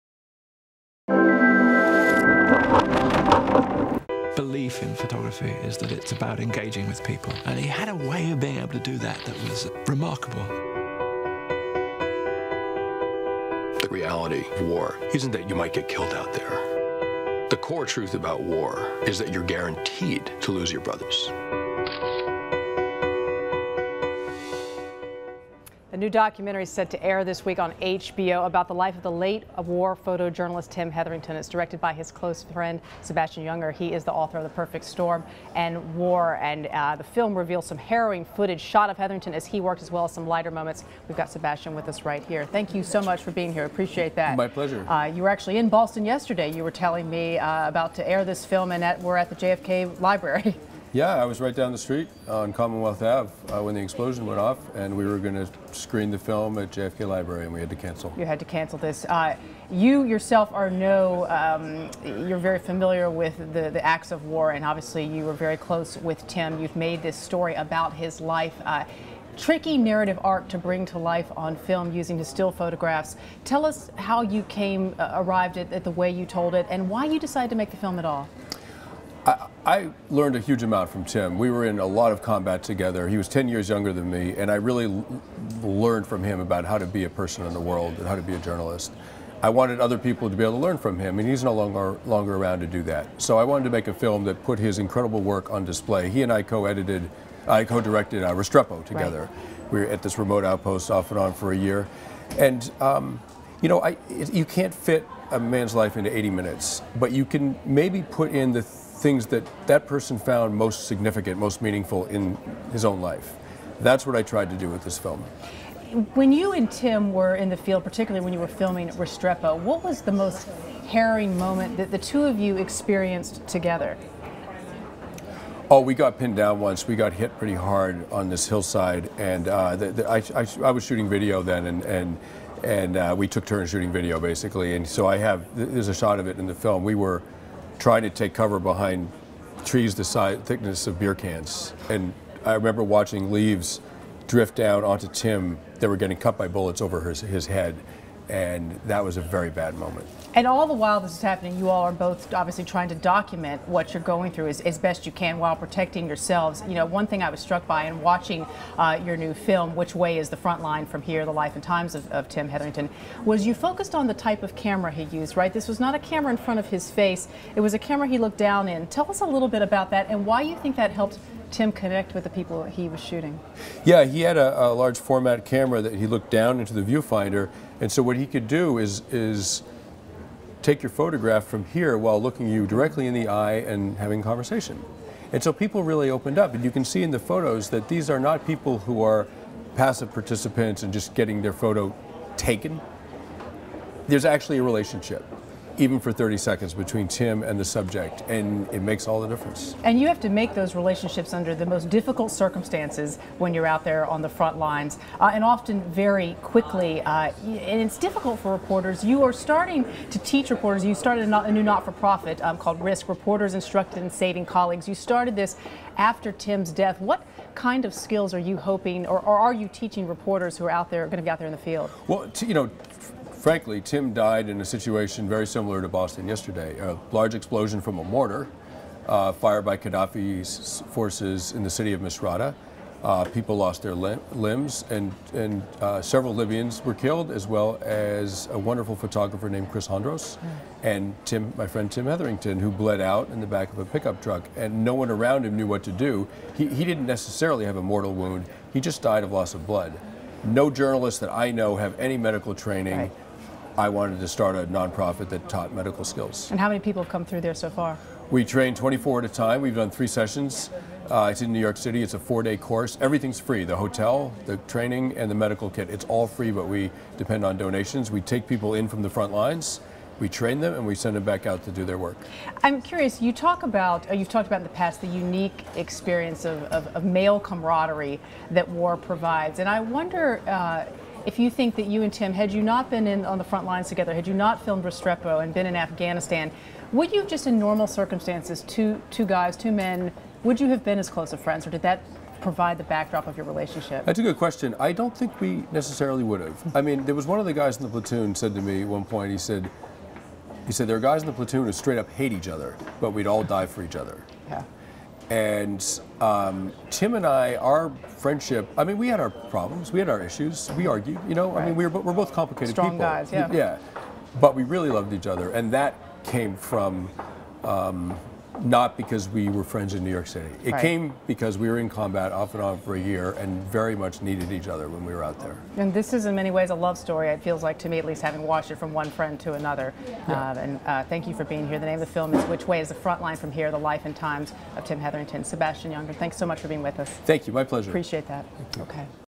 Sebastian Junger Discusses New HBO Documentary
Just a few months after being nominated for 2011 Oscar for Best Documentary, 'Restrepo' co-director Tim Hetherington died covering the uprisings in Libya. Sebastian Junger joins Lunch Break with a look at "Which Way Is the Front Line From Here," a new HBO documentary about Hetherington. Photo: Getty Images.